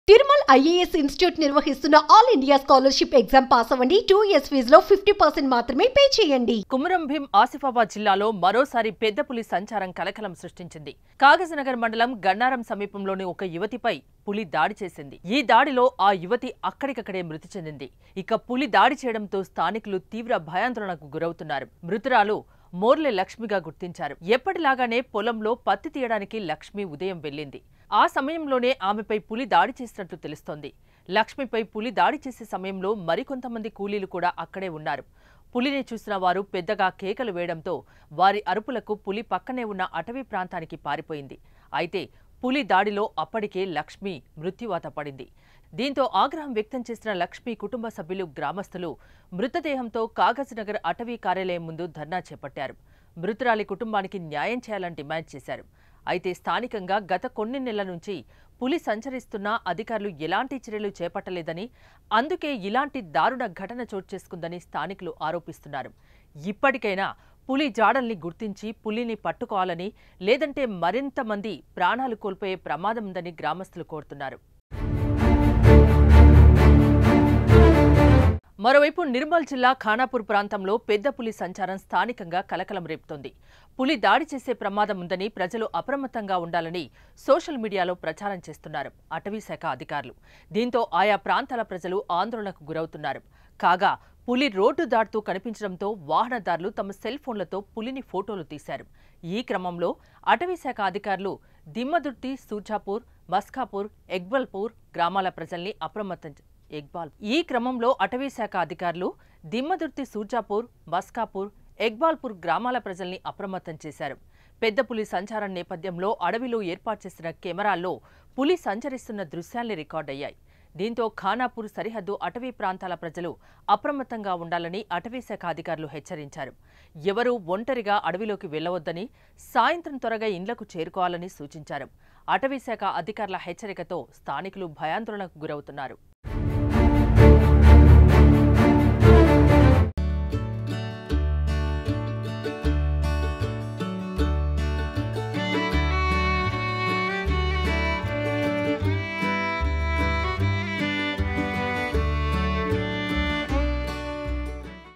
గజ గజ వణికిస్తున్న పులి..| Tiger A-t-tack On Women In Asifabad Dist
Watch► ►గజ గజ వణికిస్తున్న పులి..| Tiger A-t-tack On Women In Asifabad Dist |  @sumantvbhainsa